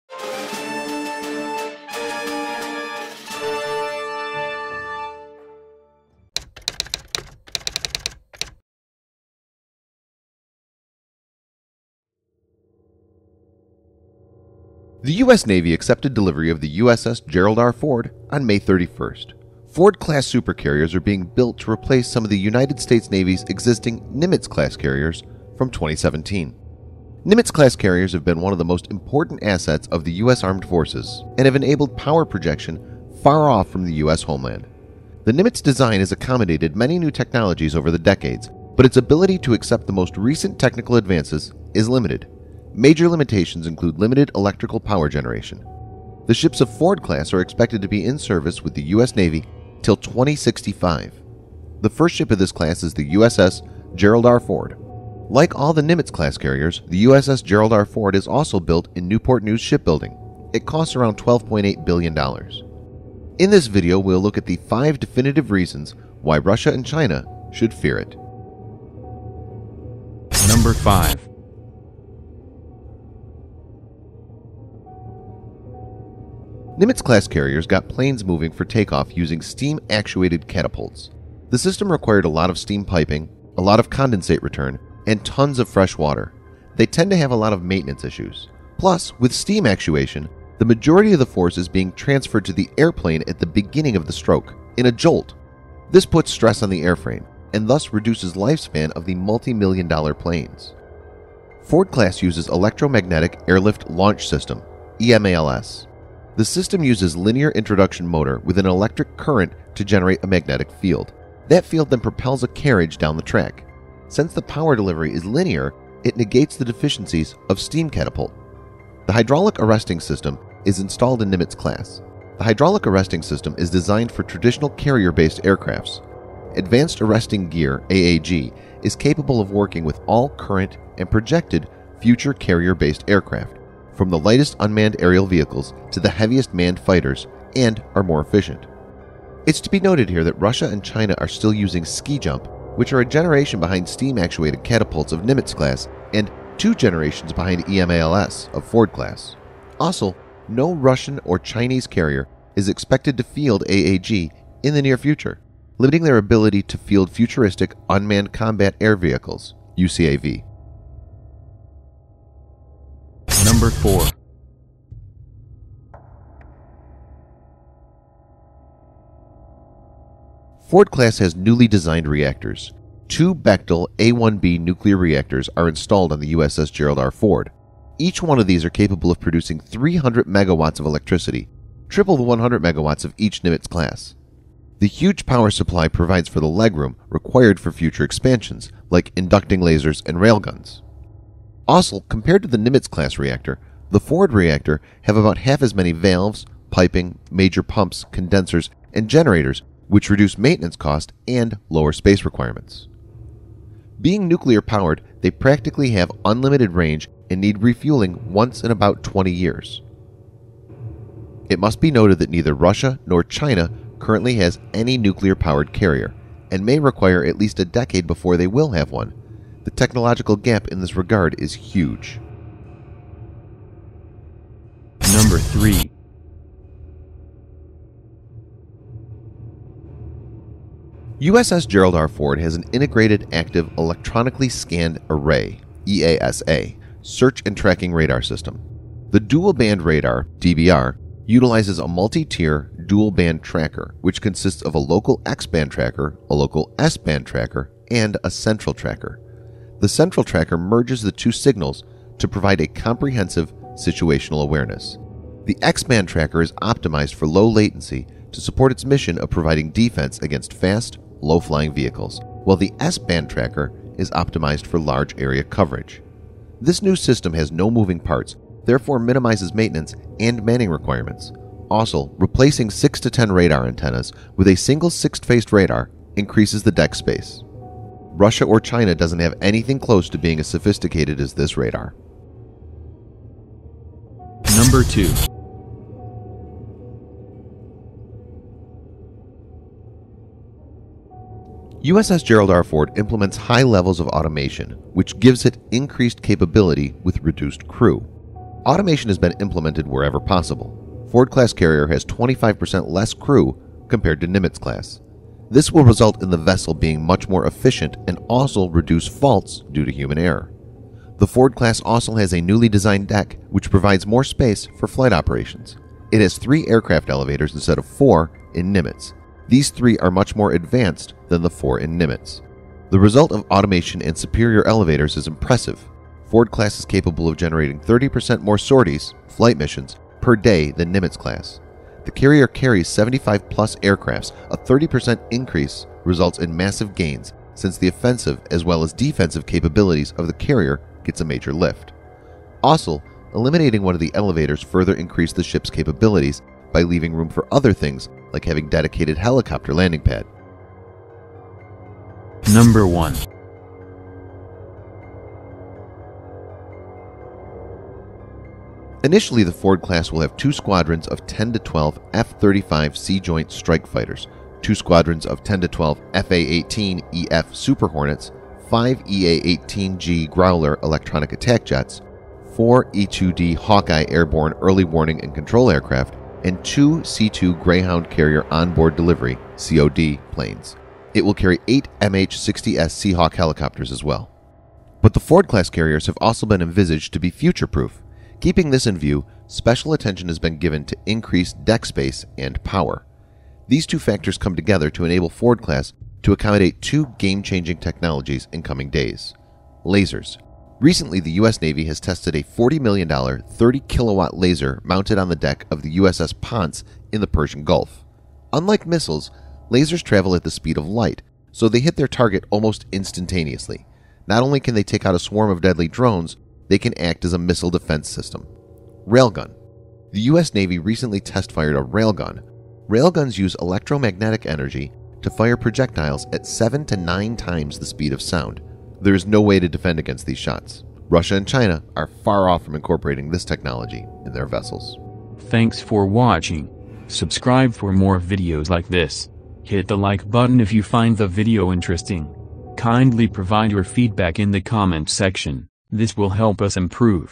WHY RUSSIA & CHINA SHOULD FEAR USS GERALD R. FORD? TOP 5 DEFINITIVE REASONS
INTRODUCTION:

The US Navy accepted delivery of the USS Gerald R. Ford on May 31st.

Ford class super-carriers are being built to replace some of the United States Navy's existing Nimitz-class carriers from 2017.

The ships of Ford class are expected to be in service with the US Navy till 2065.
The first ship of this class is USS Gerald R. Ford. Like all the Nimitz class carriers, the USS Gerald R. Ford is also build in Newport News Shipbuilding. It costs around  $12.8 billion.

In this video we will look into the 5 definitive reasons, why Russia and China should fear it?

EMALS & AAG:

Nimitz-class carriers got planes moving for takeoff using steam-actuated catapults. The system required a lot of steam piping, a large condensate return, and tons of fresh water. They tend to have a lot of maintenance issues.

Plus, with steam-actuation, the majority of the force is being transferred to the airplane at the beginning of the stroke—in a jolt. This puts stress on an airframe, and thus reduces lifespan of the multi-million dollar planes.

Ford class uses Electromagnetic Aircraft Launch System (EMALS). The system uses linear induction motor with an electric current to generate a magnetic field. That field then propels a carriage down a track. Since the power delivery is linear, it negates the deficiencies of steam catapult.

The hydraulic arresting system is installed in Nimitz class.
Hydraulic arresting system is designed for traditional carrier based aircrafts.
Advanced Arresting Gear (AAG) is capable of working with all current and projected future carrier-based aircraft, from the lightest unmanned aerial vehicles to the heaviest manned fighters and are more efficient.

It is to be noted here that Russia and China, are still using using ski jump, which are generation behind steam-actuated catapults of Nimitz class, and two generation behind EMALS of Ford class.

Also no Russian or Chinese carrier is expected to field AAG in near future, limiting their ability to field futuristic Unmanned Combat Air Vehicle (UCAV).

MORE POWER:

Ford class has newly designed reactors.
2 Bechtel A1B nuclear reactors are installed on USS Gerald R. Ford. Each one these are capable of producing 300 MW of electricity, triple the 100 MW of each Nimitz-class.

The huge power supply provides for the legroom, required for future expansions like inducting Lasers and Rail Guns.

STATE OF ART RADAR:

USS Gerald R. Ford has an integrated Active Electronically Scanned Array (AESA) search and tracking radar system. The dual-band radar (DBR) utilizes a multi tier, dual-band tracker, which consists of a local X band tracker, a local S band tracker, and a central tracker. The central tracker merges the two signals to provide a comprehensive situational awareness.

HIGHER FREQUECY OF AIRCRAFT LAUNCH:

USS Gerald R. Ford implements high levels of automation, which gives it, increased capability with reduced crew.
Automation has been implemented wherever possible.
Ford-class carrier has 25% less crew compared to Nimitz class.

This will result in the vessel being much more efficient, and also reduce faults due to human error.

The Ford class also has a newly designed deck, which provides more space for flight operations. It has 3 aircraft elevators instead of 4 in Nimitz. These 3 are much more advanced than the 4 in Nimitz.

The result of Automation & superior elevators is impressive.
Ford class is capable of generating 30 % more sorties (flight missions) per day than Nimitz‐class.

LASERS & RAIL GUN

Initially the Ford class will have 2 squadrons of 10 to 12 F-35C Joint Strike Fighters, 2 squadrons of 10 to 12 F/A-18E/F Super Hornets, 5 EA-18G Growler electronic attack jets, 4 E-2D Hawkeye airborne early-warning and control aircraft, and 2 C-2 Greyhound carrier onboard delivery (COD) planes. It will carry 8 MH-60S Seahawk helicopters as well.

These two factors come together to enable Ford class to accommodate two game changing technologies in coming days:
1. Lasers: Recently the US Navy has tested a $40 million, 30-kilowatt laser mounted on the deck of the USS Ponce in the Persian Gulf. Unlike missiles, lasers travel at the speed of light, so they hit their target almost instantaneously. Not only can they take out a swarm of deadly drones, they can act as a missile defense system.
2. Rail Gun: The US Navy recently test fired a railgun. Railguns use electromagnetic energy to fire projectiles at 7 to 9 times the speed of sound. There is no way to defend against these shots.

Russia & China are far off from incorporating this technology in their vessels.